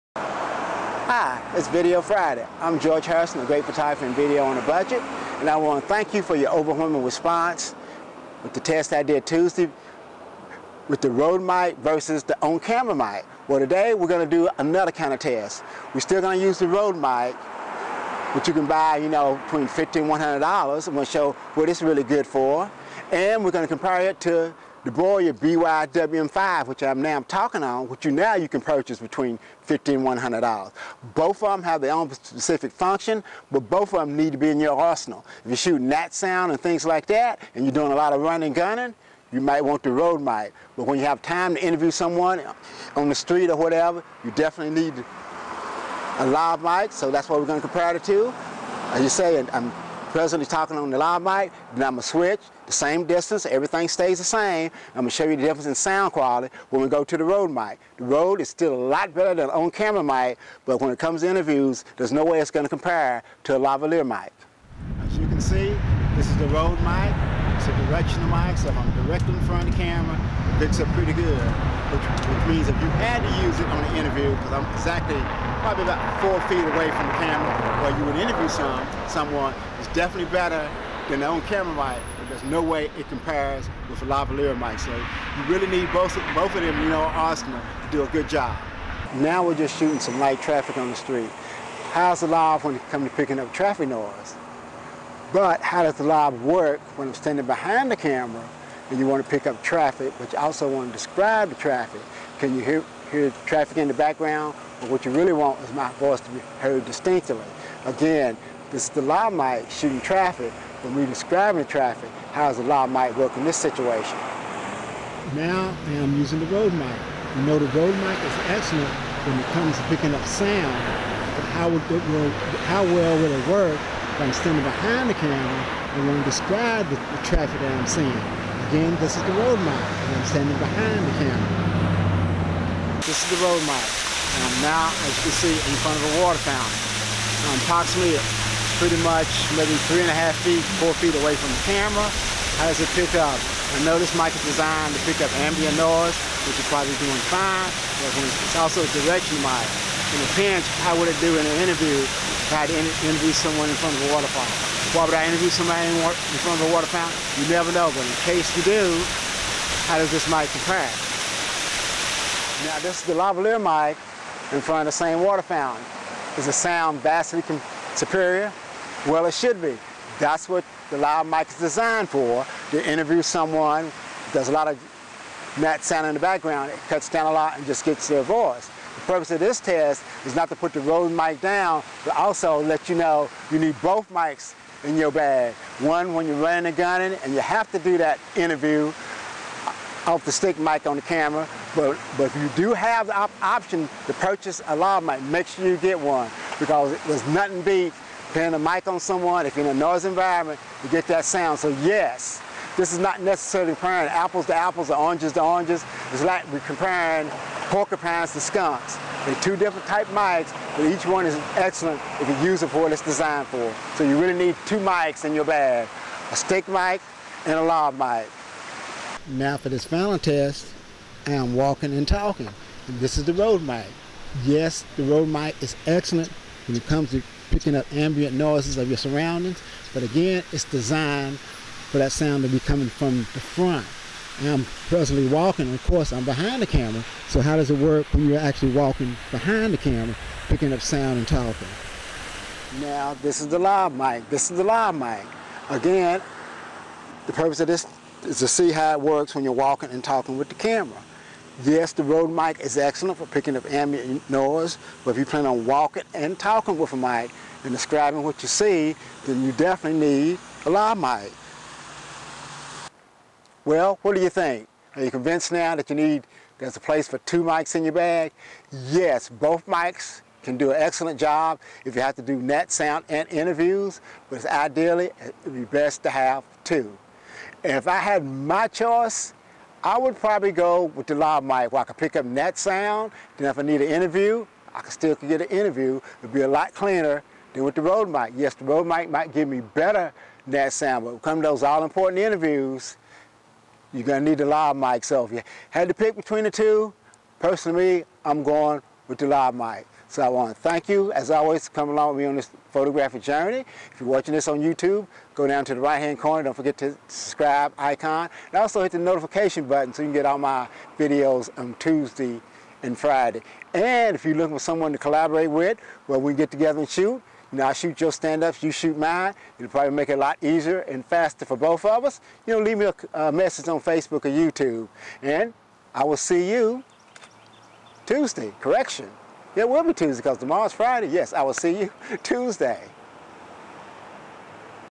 WHY YOU NEED A LAV AND STICK MIC IN YOUR CAMERA BAG
This is an illustration to why you need a stick mic and a Lav mic in your camera bag. Both mics or good and can be used for both jobs.
In a perfect world if possible it is best to have both.